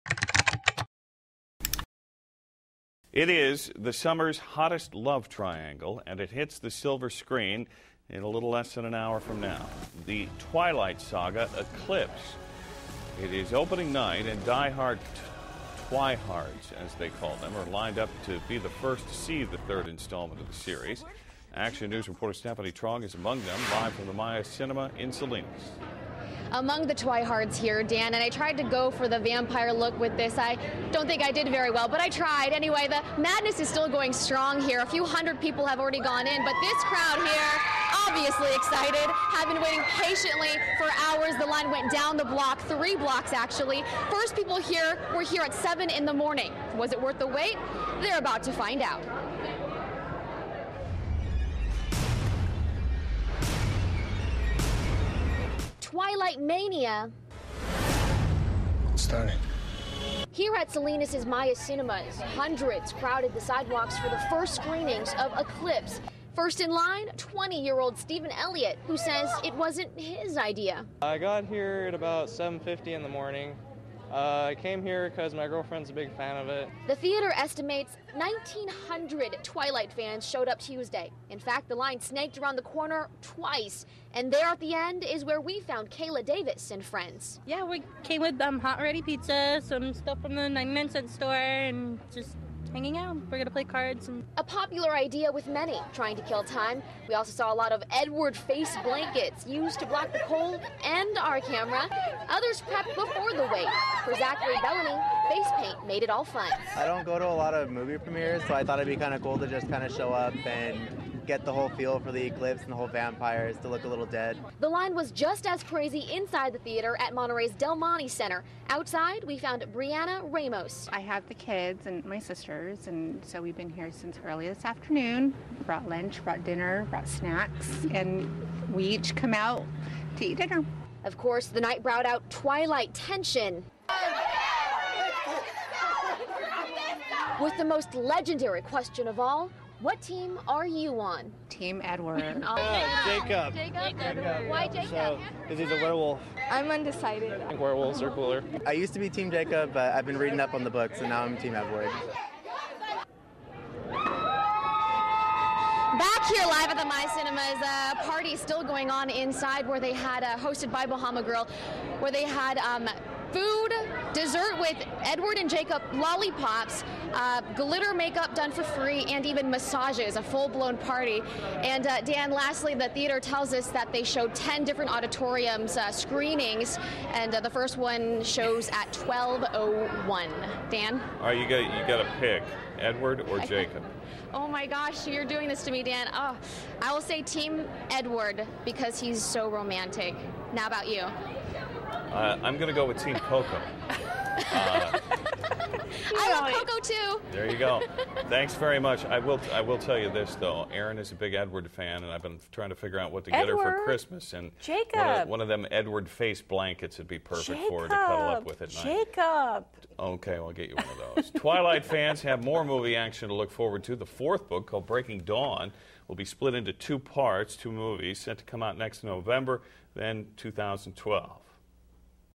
Opening Night For Twilight
Twilight fans were lining up for opening night of the newest installment, Eclipse.